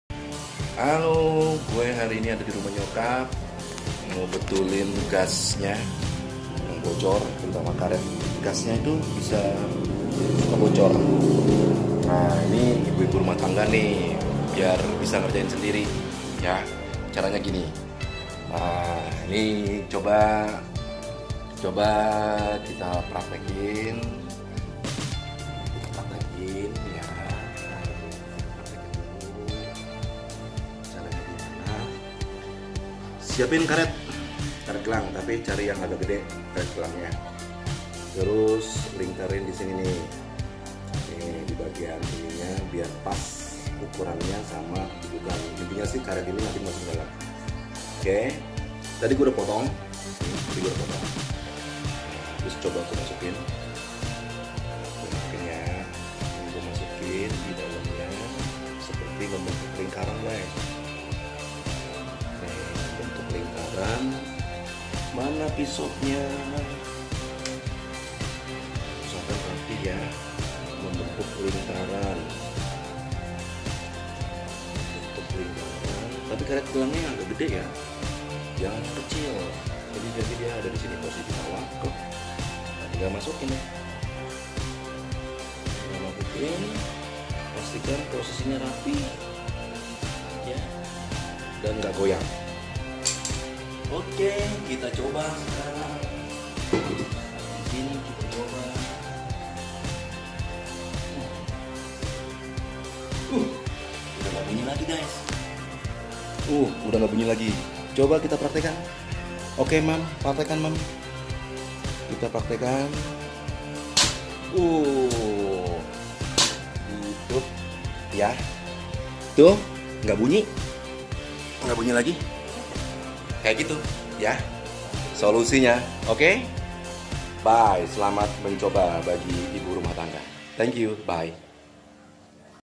Solusi Perbaiki Tabung Gas Bocor !!!!
Solusi buat mak2 gaul yang sering punya kendala tabung gas nya bocor, terutama pada bagian karet di kepala tabung gas,
Cara yang mudah dan sederhana kayak gini nZiiiiiiirrrrrrr. selamat mencoba, semoga bermanfaat.
salam 2135 NNjooosssss
for waching